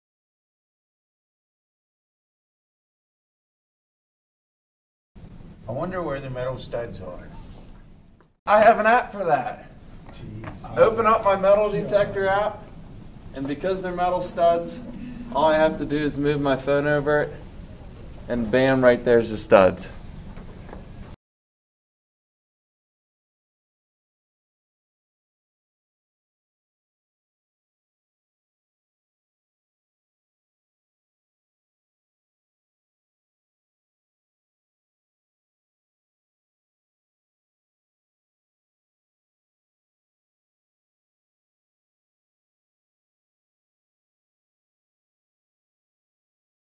High-Tech Maintenance Department
Union School District's Maintenance Department goes High-Tech